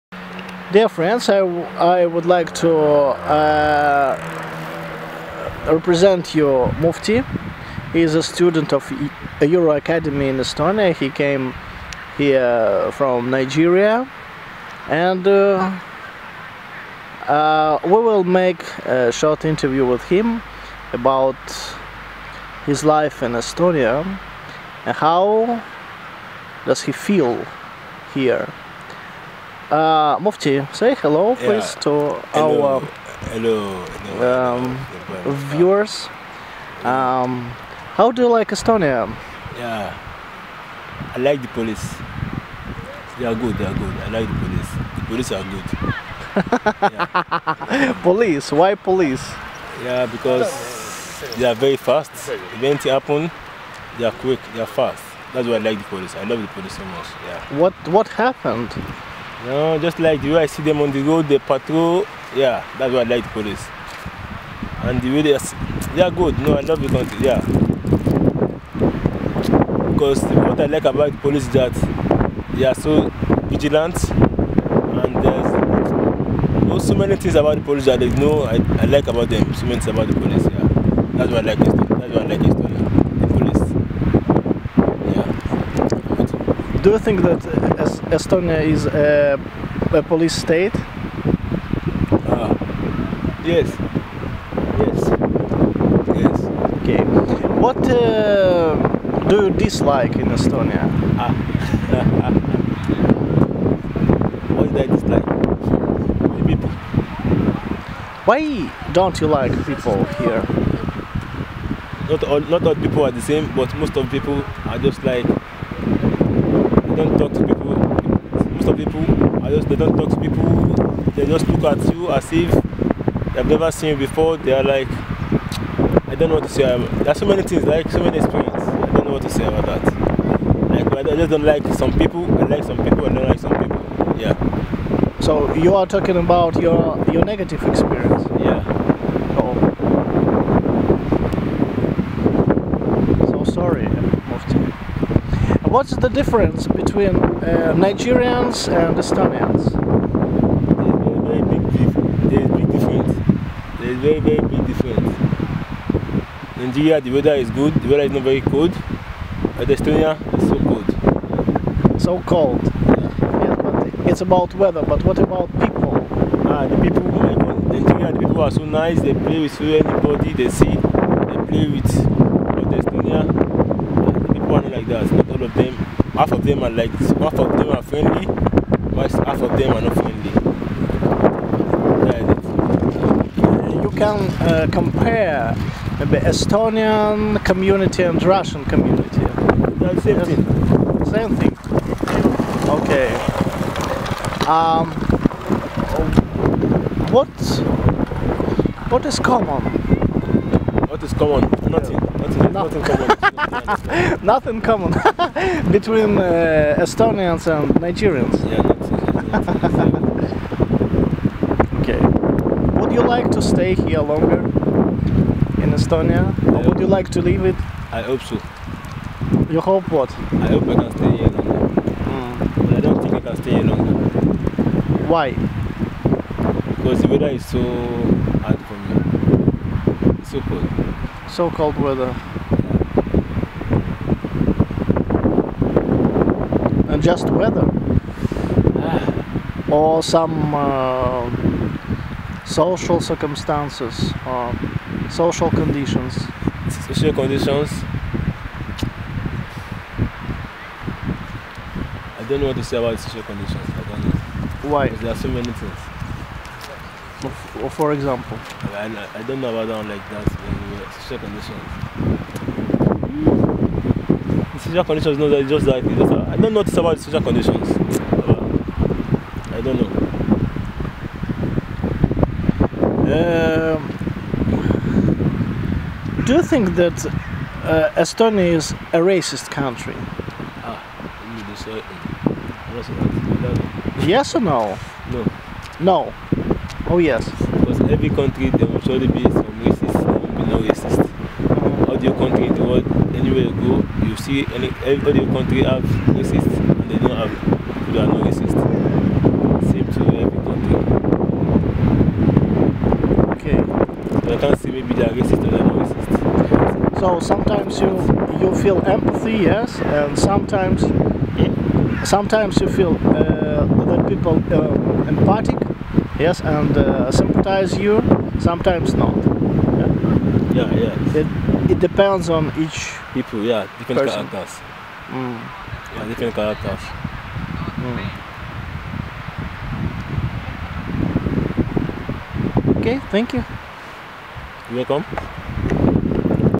Nigerian student: Estonia is a police state
An interview with a Nigerian student Muftau Salau about his life and experience in Estonia. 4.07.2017. Tallinn.